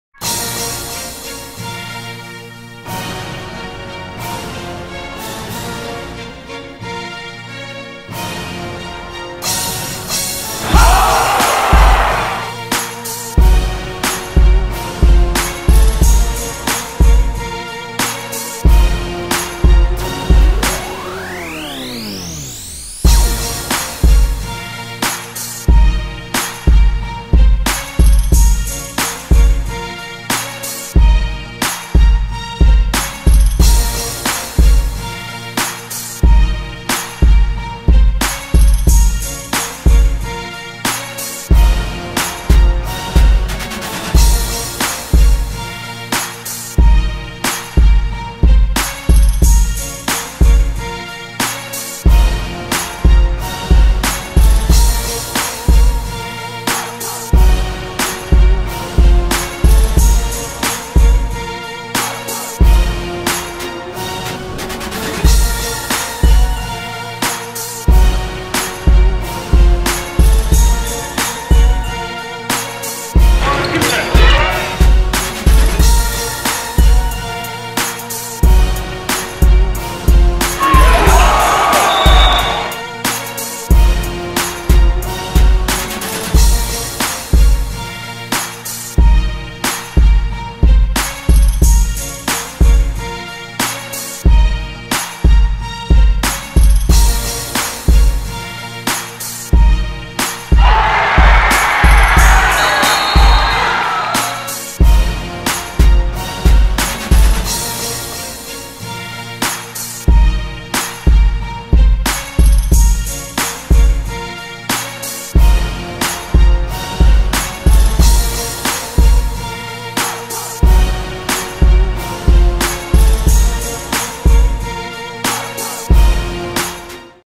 Top Florida Talent Shows Out at Hoops with a Heart
Some of the best players in Florida took the court at Hoops with a Heart.